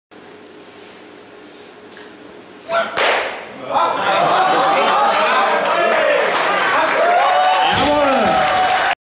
Nina Schroth 2. Versuch Reissen 97kg
Videos der Begegnung AC 1892 Mutterstadt - Oder-Sund-Team in der 1.Gewichtheber-Bundesliga vom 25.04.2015